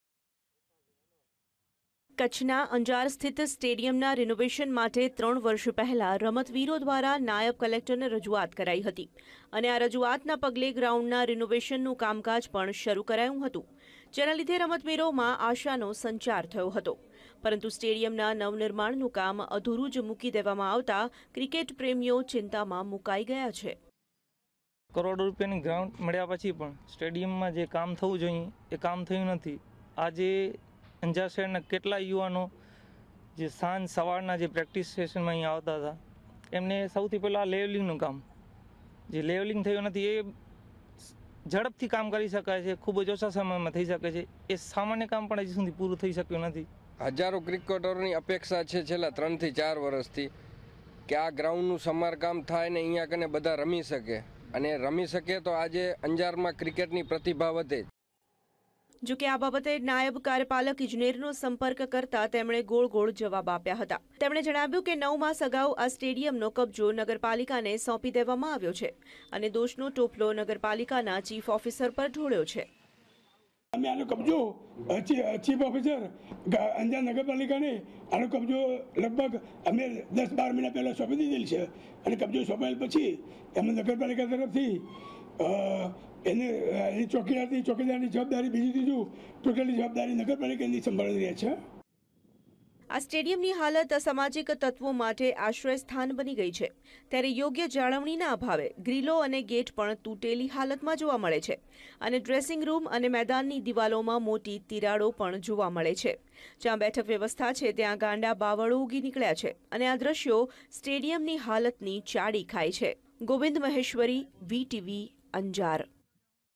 ANJAR STADIUM IN BAD CONDITION-VTV
સમગ્ર કચ્છ જિલ્લામાં જે ગ્રાઉન્ડની રચના આદર્શ ગણવામાં આવે છે..તેવા અંજારના સ્ટેડિયમની હાલત દિનપ્રતિદીન કથળી રહી છે. આ મેદાનના આધુનિકરણ માટે સવા કરોડની માતબર રકમ મંજુર તો થઇ
છે.પણ તેનું કામ શરૂ થયું નથી.જેને કારણે અનેક સવાલો ઉઠી રહ્યા છે.